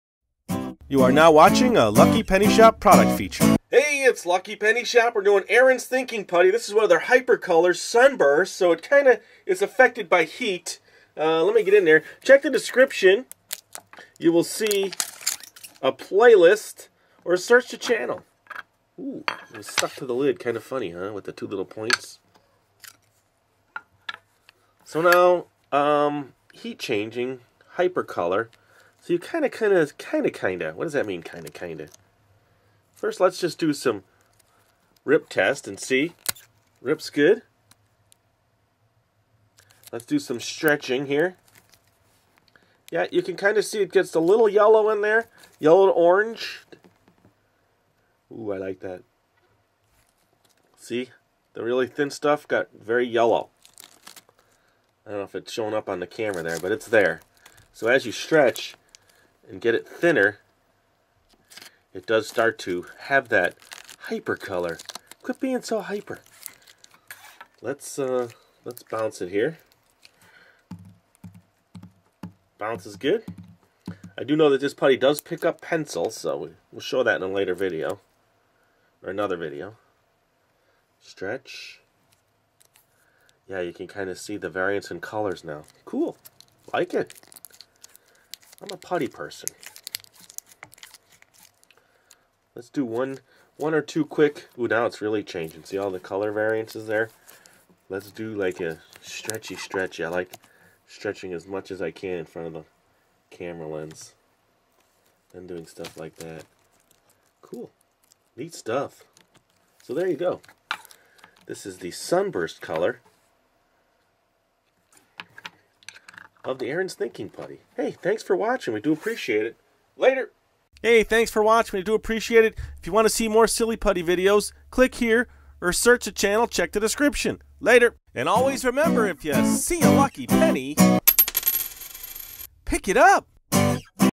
Crazy Aaron's Thinking Putty - Sunburst Hyper Color - Reacts To Heat!
Watch our product feature video for Crazy Aaron's Thinking Putty - Sunburst Hyper Color - Reacts To Heat.